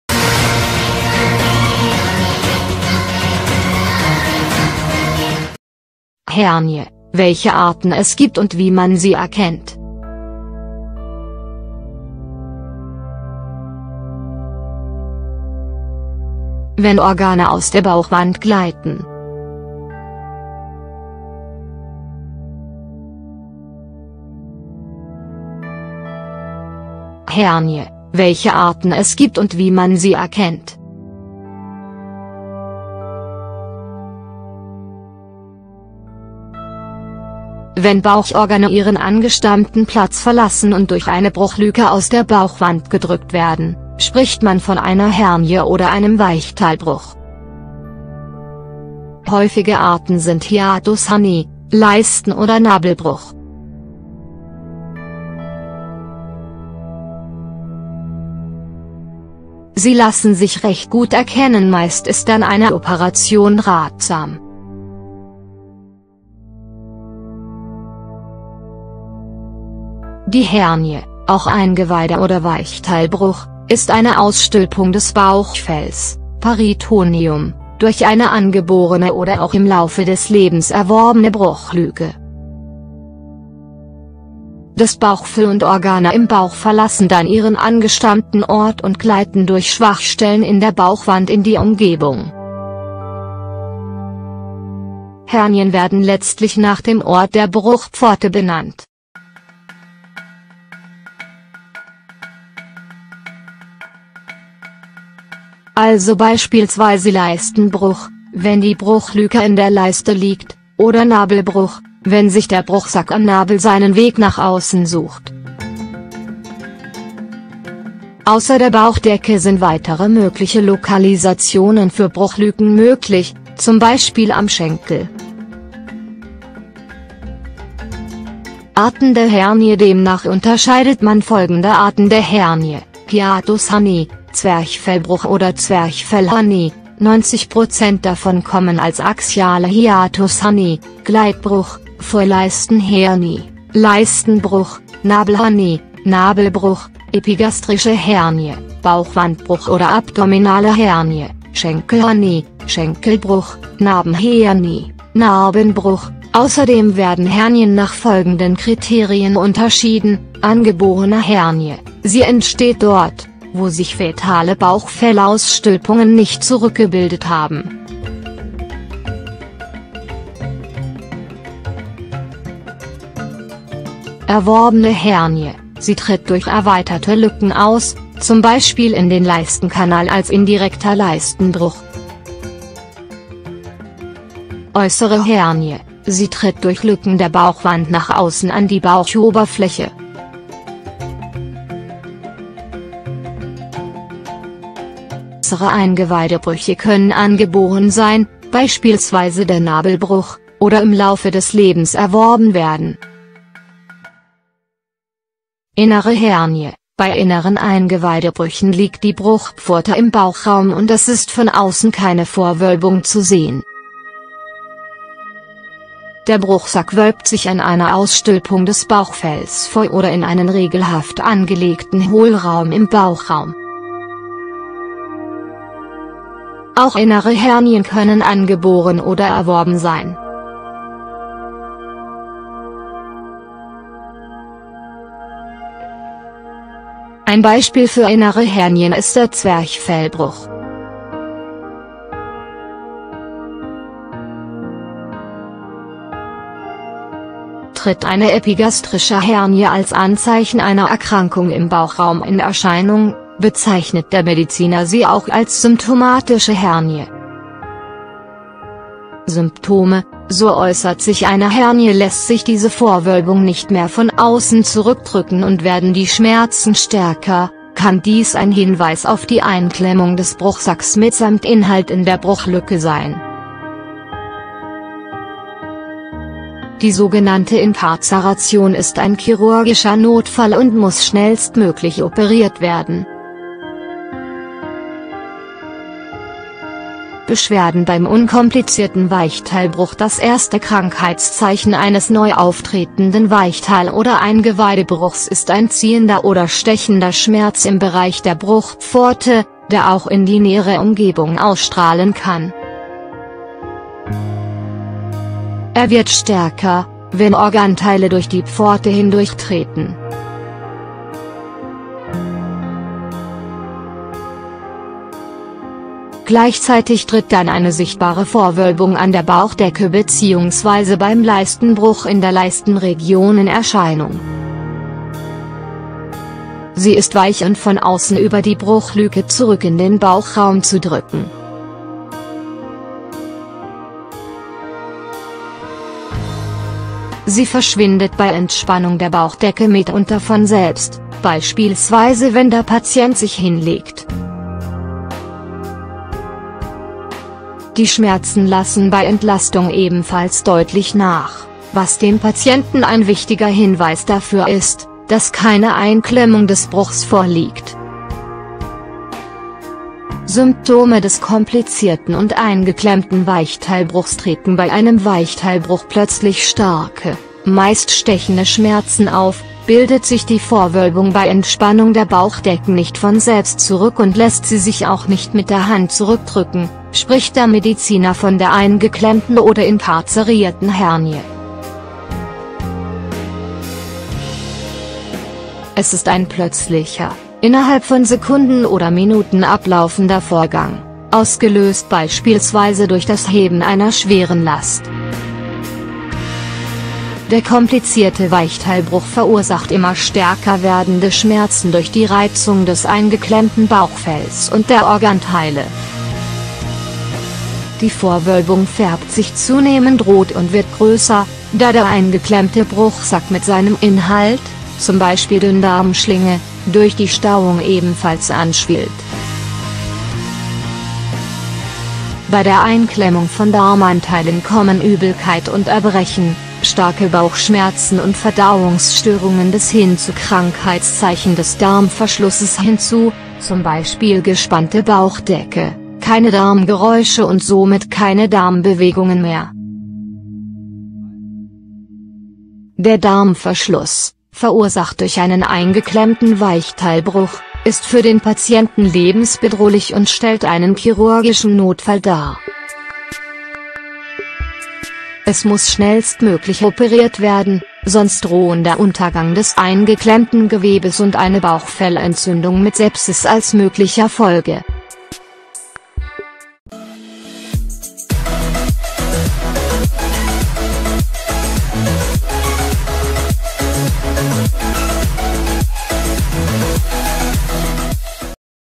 Hernie: Welche Arten es gibt und wie man sie erkennt
Wenn Bauchorgane ihren angestammten Platz verlassen und durch eine Bruchlücke aus der Bauchwand gedrückt werden, spricht man von einer Hernie oder einem Weichteilbruch. Häufige Arten sind Hiatushernie, Leisten- oder Nabelbruch. Sie lassen sich recht gut erkennen – meist ist dann eine Operation ratsam.